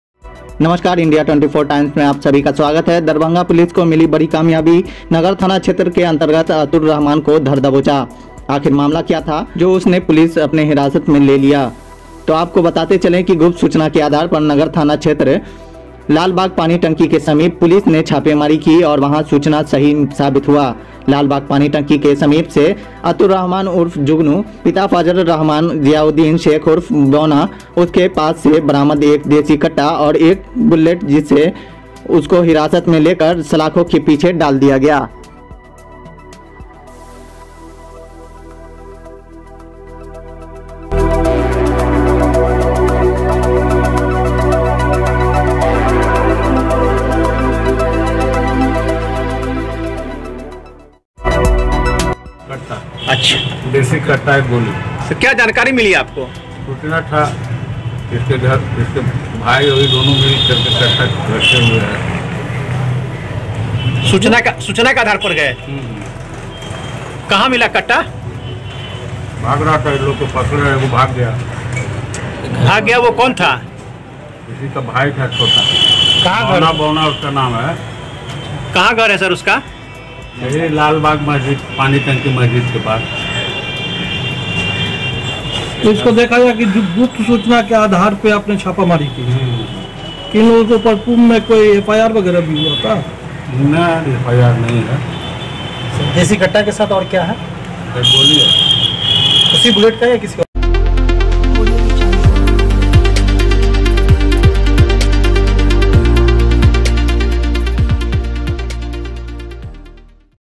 गुप्त सूचना के आधार पर दरभंगा पुलिस को मिली बड़ी कामयाबी नगर थाना अंतर्गत देसी कट्टा और बुलेट बरामद।
नमस्कार दोस्तों एक बार आप फिर आप सभी का स्वागत है दरभंगा पुलिस को मिली बड़ी कामयाबी नगर थाना क्षेत्र के अंतर्गत अतुर रहमान को धर दबोचा आखिर मामला क्या था जोर से पुलिस अपने हिरासत में ले ली तो आपको बताते चलें गुप्त सूचना के आधार पर नगर थाना क्षेत्र लालबाग पानी टंकी के समीप पुलिस ने छापेमारी की और वह सूचना सही साबित हुआ लालबाग पानी टंकी के समीप से अतुर रहमान उर्फ जुगनू, पिता फजलर रहमान, जियाउद्दीन शेख उर्फ बौना उसके पास से बरामद एक देशी कट्टा और एक बुलेट जिस को हिरासत में लेकर सलाखों के पीछे डाल दिया गया